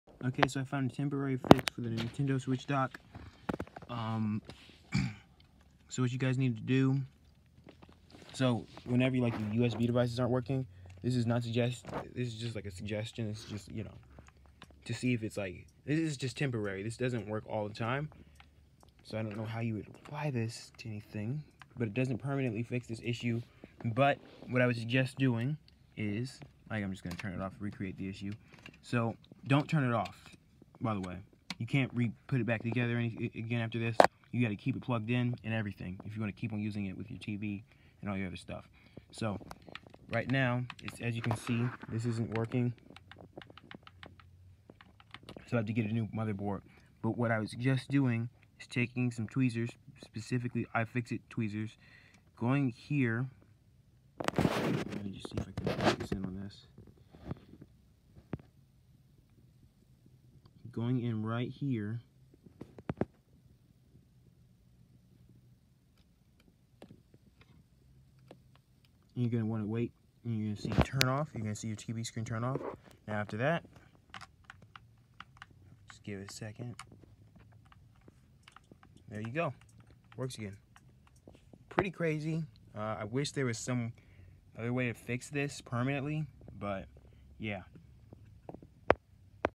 How to fix nintendo switch dock USB ports not working
Reset the cap and chip